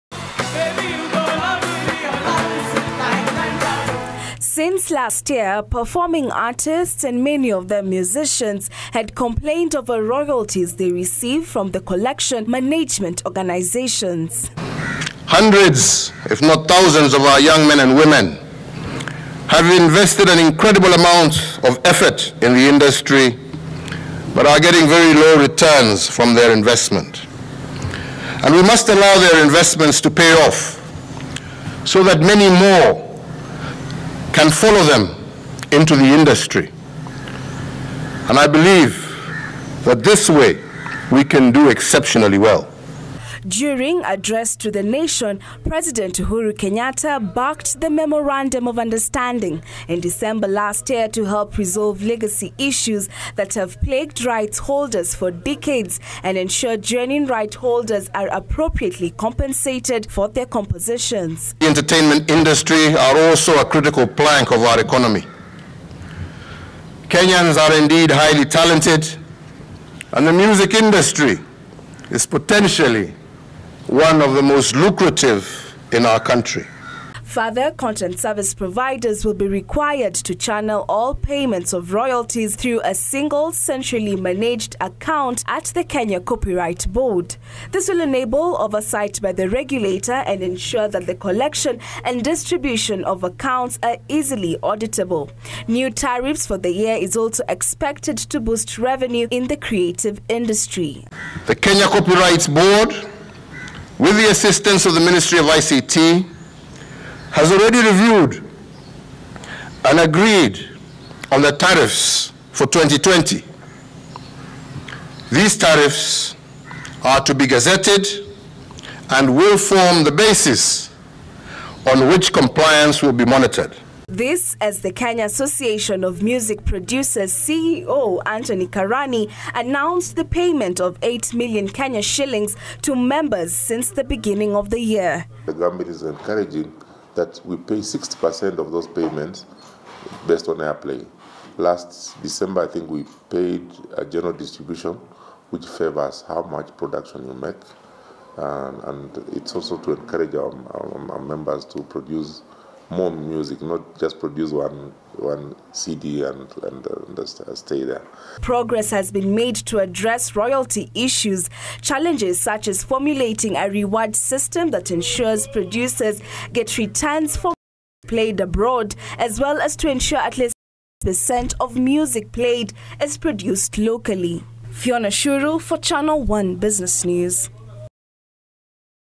President addresses royalty payments
Since last year performing artists and many of them musicians have complained over royalties  they received from  thev  collection management organisations.